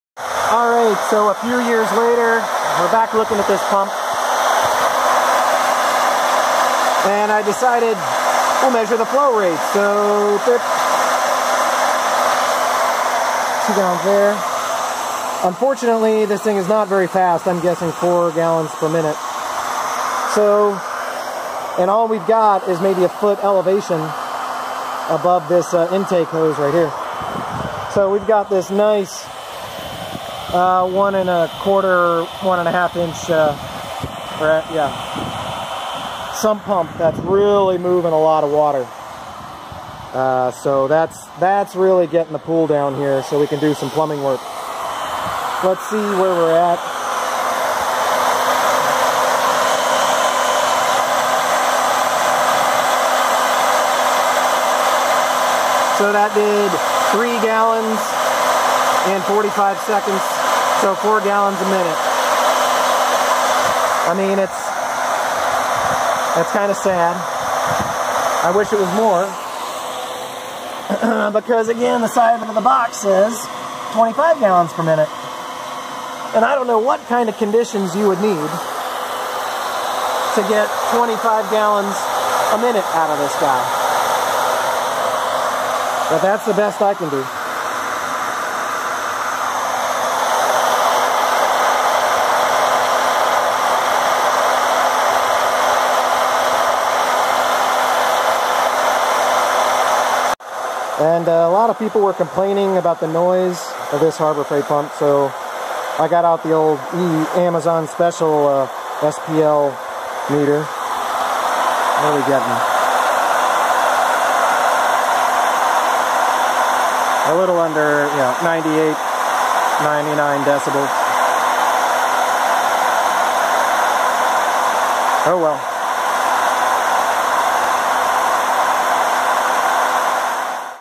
Harbor Freight Portable Utility Water Pump 65836 Update on flow rate and loudness
I don't think I've used this pump since my original video in 2013, but since I had it out again, I figured an update was warranted given feedback on the first one.
Harbor Freight does not sell this anymore, but they do sell a similar model as the DRUMMOND 1/2 HP Cast Iron Transfer Pump, item 63316. It has the exact same power and performance specs.
In short: I was way off on the effectiveness of this pump. It will only move 4 GPM; I don't know how you would get the 25 GPM as advertised. Also, according to my SPL meter, it gets up to 100dB at the pump.
We switched to a submersible pump to do the rest of the work. It moved exponentially more water at 1/4 the power and with zero noise (since it was under 4 feet of water.